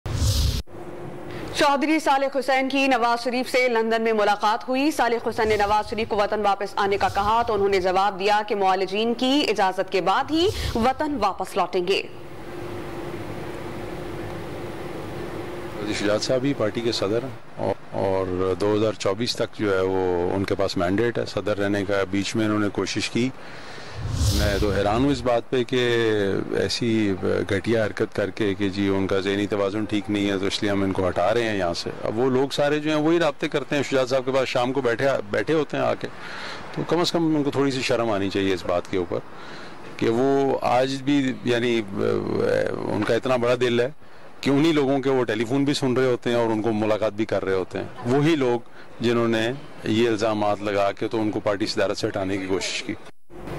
Chaudhry Salik Hussain Say Nawaz Sharif Ki London Main Mulaqat | Breaking News | Dawn News
چوہدری سالک حسین سے نوازشریف کی لندن میں ملاقات